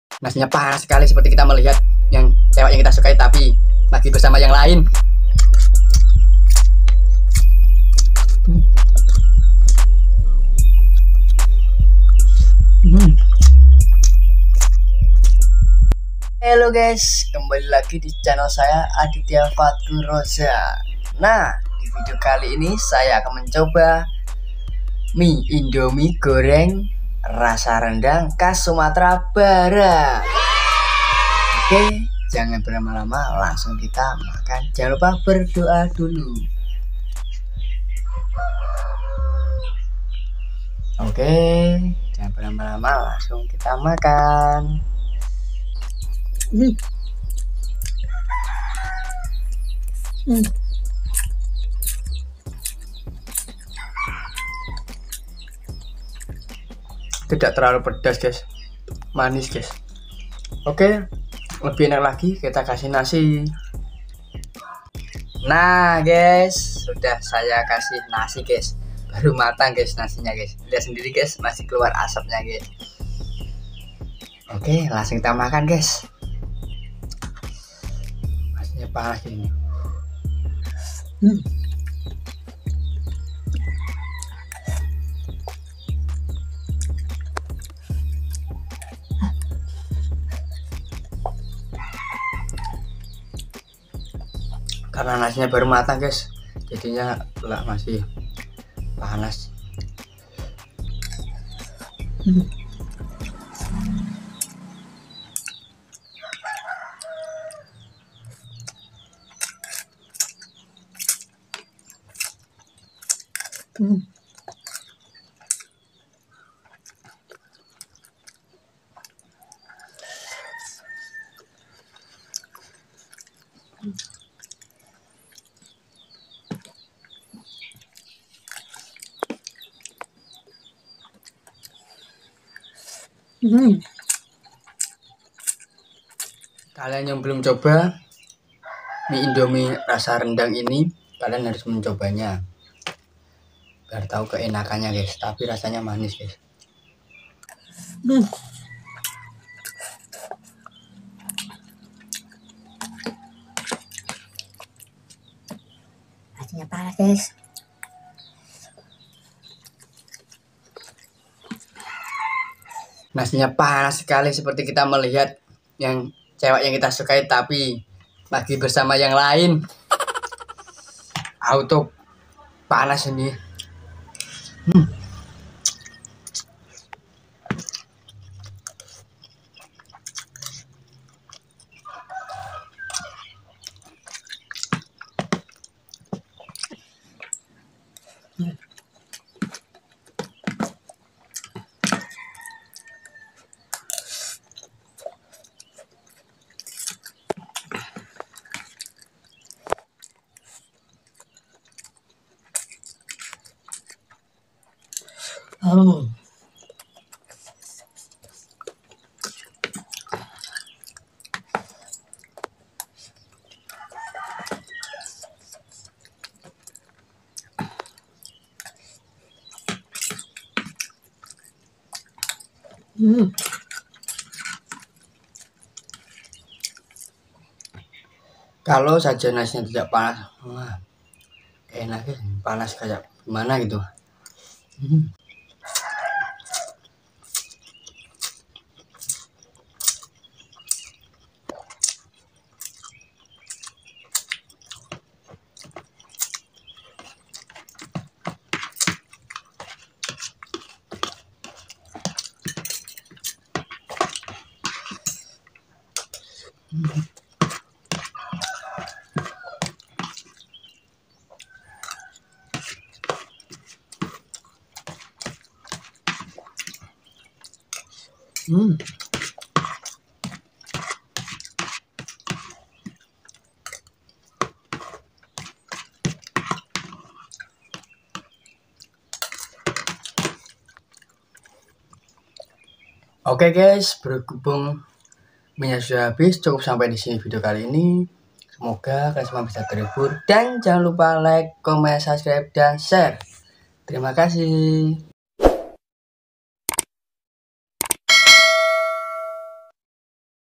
MUKBANG INDOMIE GORENG RASA RENDANG KHAS SUMATERA BARAT ‼️
selamat menonton semoga bisa terhibur dengan Vidio kali ini dari kita.